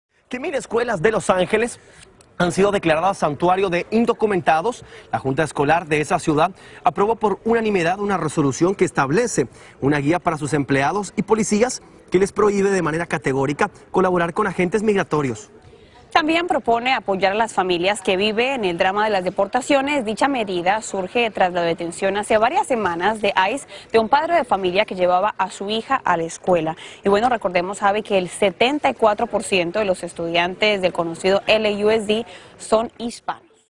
Junta escolar de Los Ángeles aprueba que 1,000 escuelas de la ciudad sean declaradas ''santuario''
La medida ayudará a proteger a los estudiantes y sus familias que tengan algún riesgo migratorio, como ocurrió con latino detenido en la puerta de la escuela de su hija cuando llegaba a dejarla.